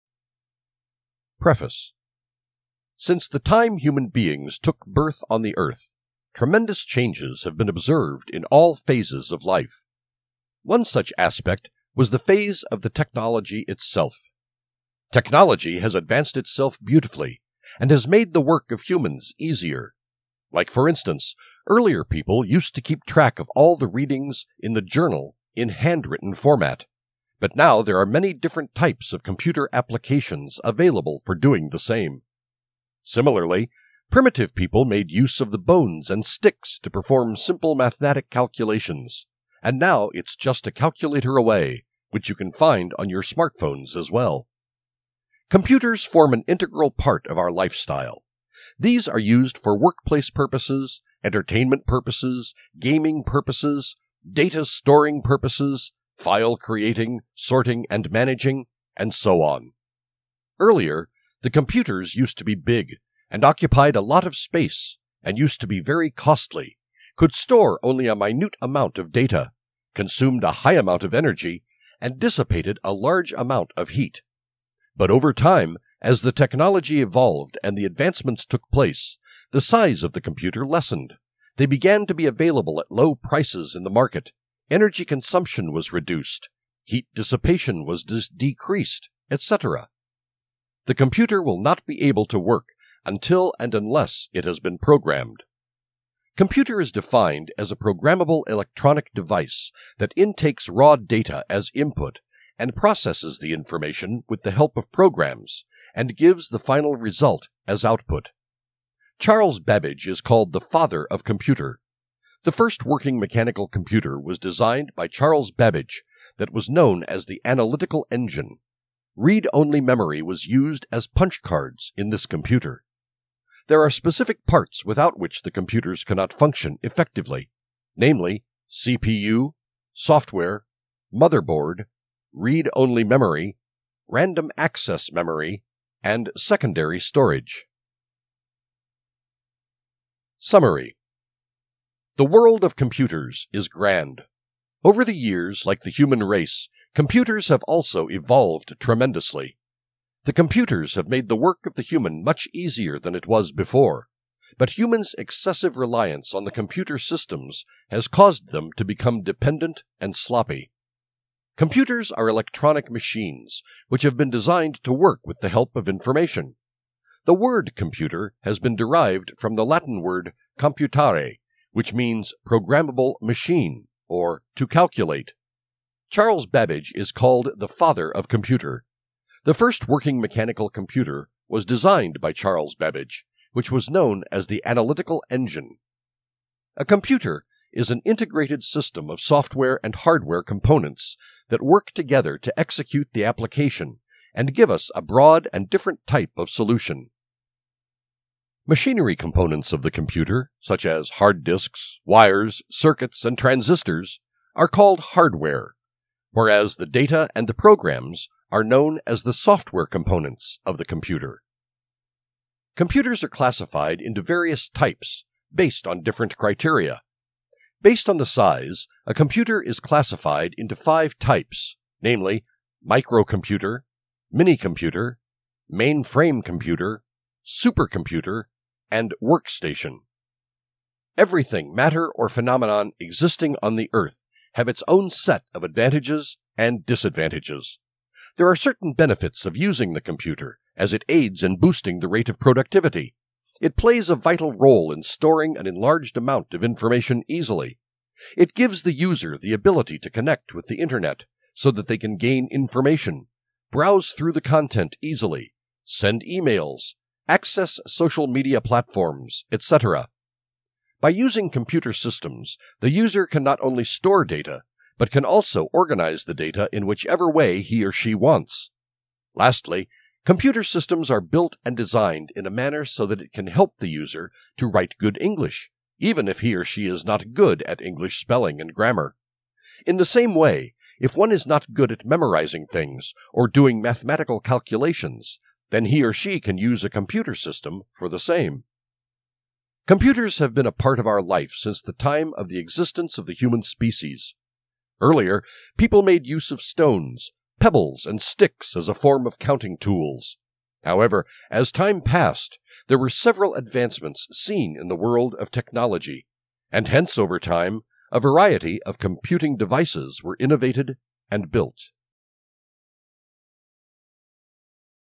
History of Computers
Since the time human beings took birth on the earth, tremendous changes have been observed in all phases of life. One such aspect was the phase of the technology itself. Technology has advanced itself beautifully and has made the work of humans easier. Like for instance, earlier people used to keep track of all the readings in the journal in handwritten format, but now there are many different types of computer applications available for doing the same. Similarly, primitive people made use of the bones and sticks to perform simple mathematical calculations, and now it’s just a calculator away, which you can find on your smartphones as well.